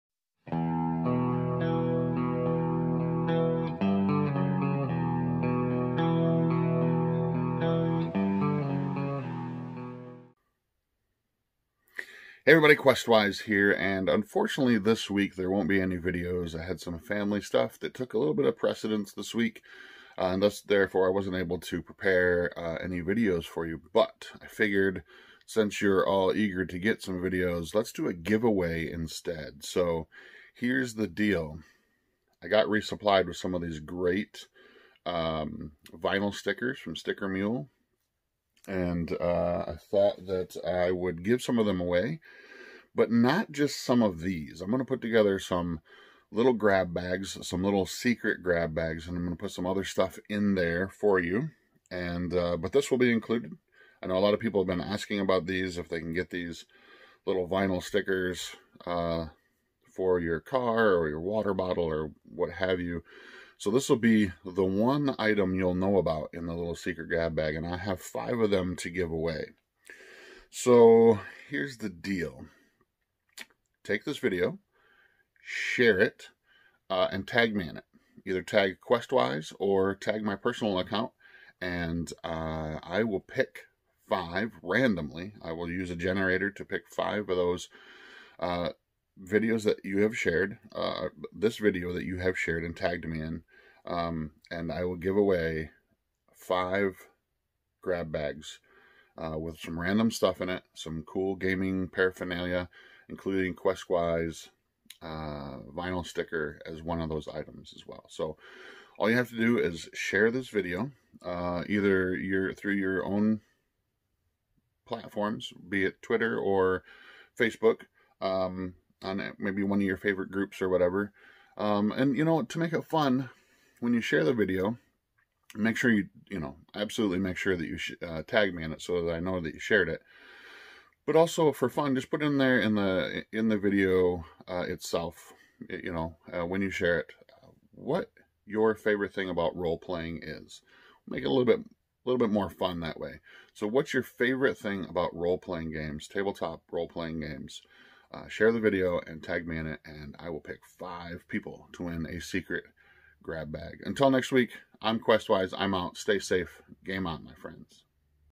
Contest Time!!!
I will draw 5 random shares to win a QuestWise Grab Bag - including a QuestWise vinyl sticker.

Contest ends June 30th, 2020